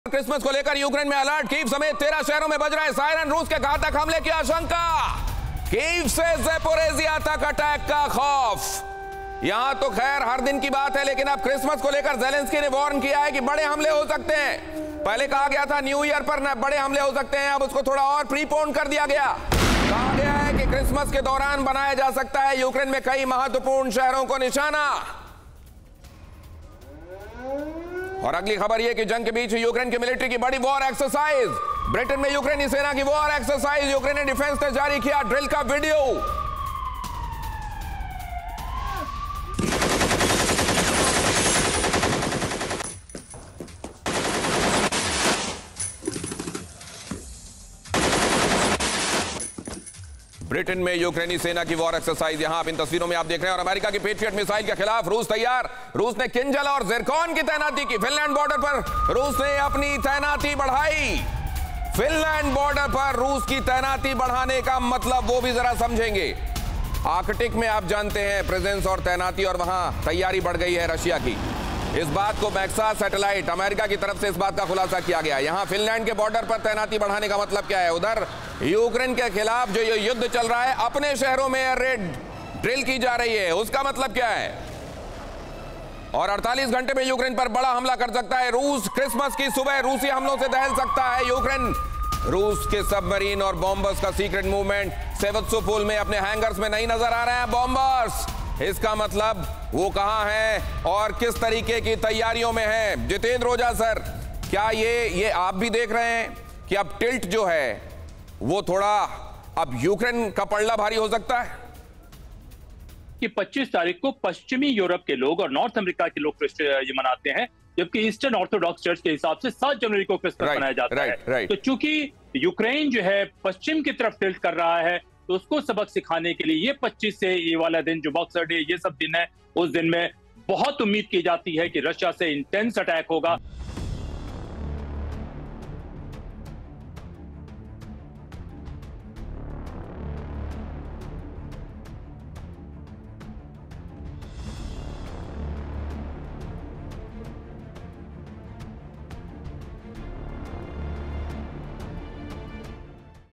Christmas को लेकर यूक्रेन में Alert | Russia Ukraine War | Putin | Zelensky | NATO
क्रिसमस (Christmas ) को लेकर यूक्रेन में अलर्ट
कीव सहित 13 शहरों में बज रहे सायरन
रूस के घातक हमले की आशंका
कीव से जेपोरिजिया तक अटैक का खौफ